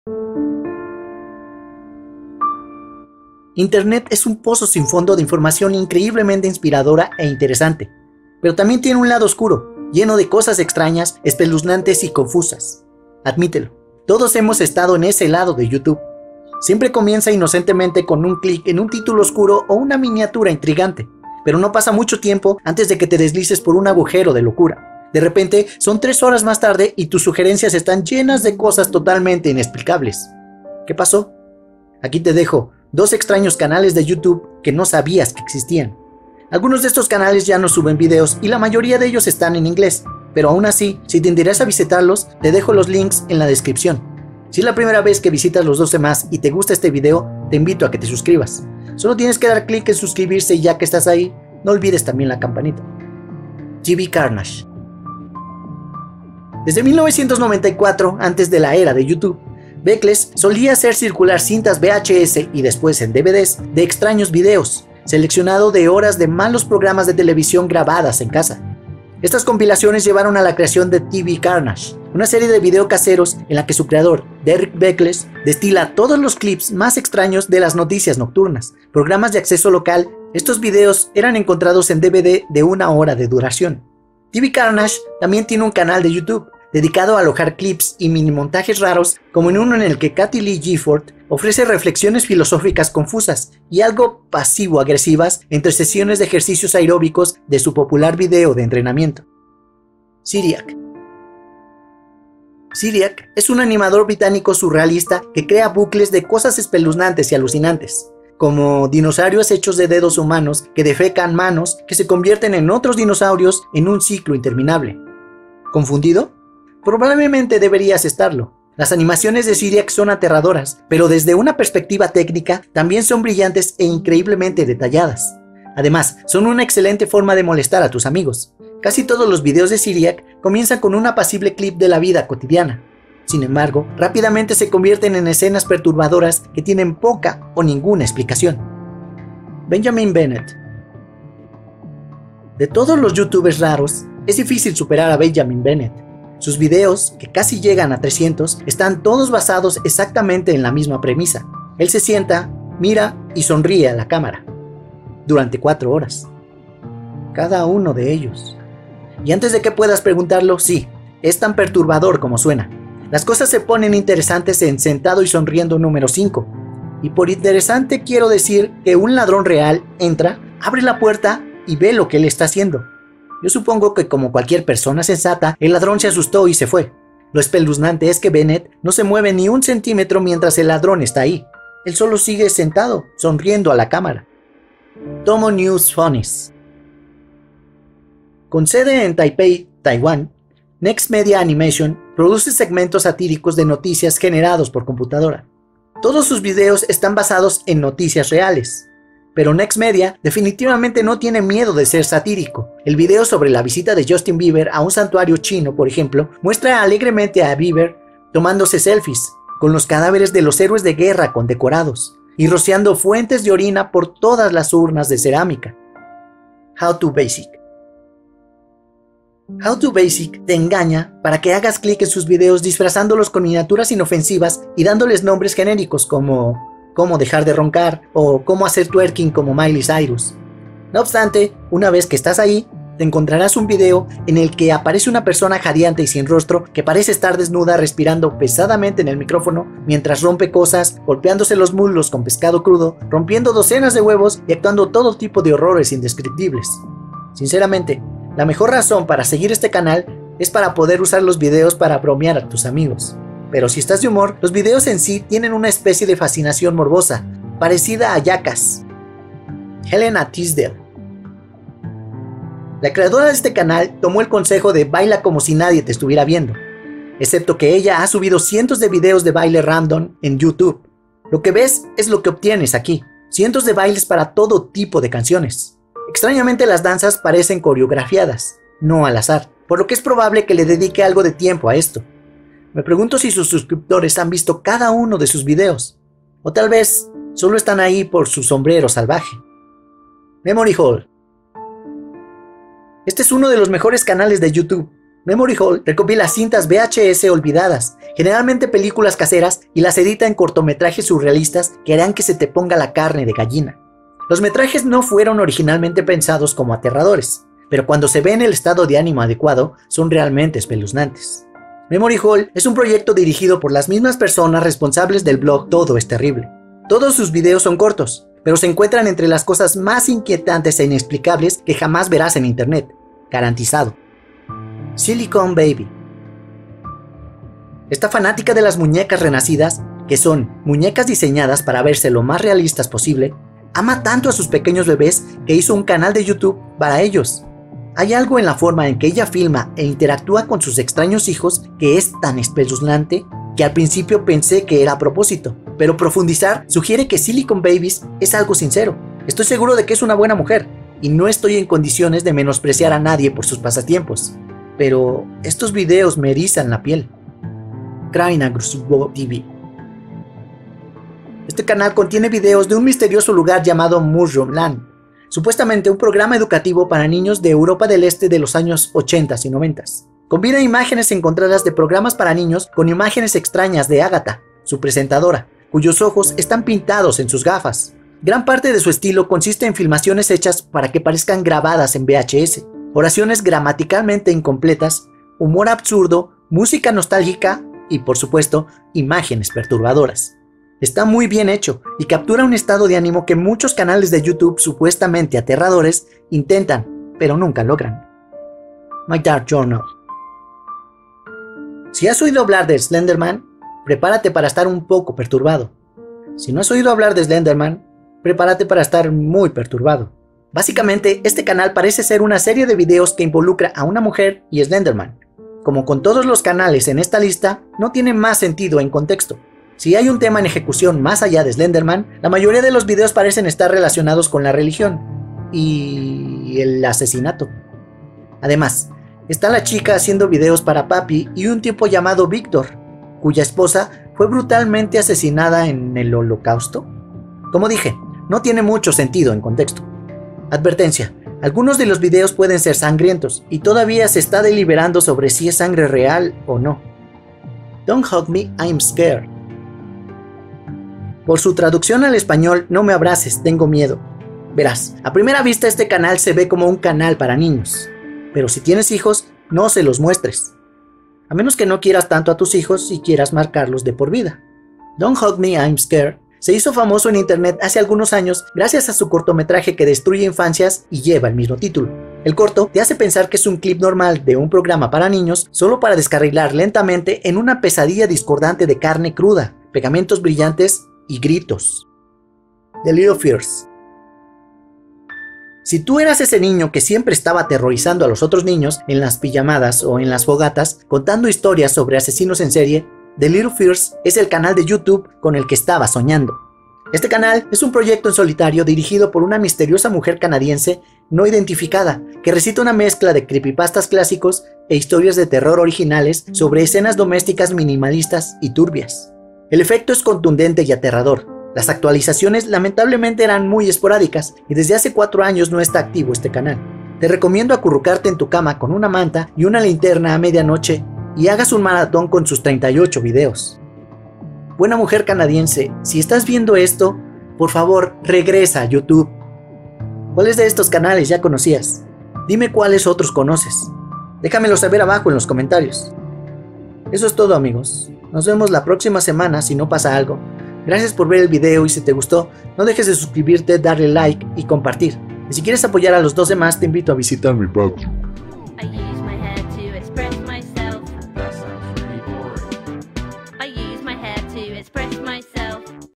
12 extraños canales de Youtube que no sabias que existian | Los 12 Mas
Internet es un pozo sin fondo de información increíblemente inspiradora e interesante. Pero, también tiene un lado oscuro, lleno de cosas extrañas, espeluznantes y confusas.
Admítelo, todos hemos estado en ese lado de YouTube.
Aquí te dejo 12 extraños canales de youtube que no sabias que existian

Si hablas otro idioma no olvides que puedes ayudarme a traducir estos videos.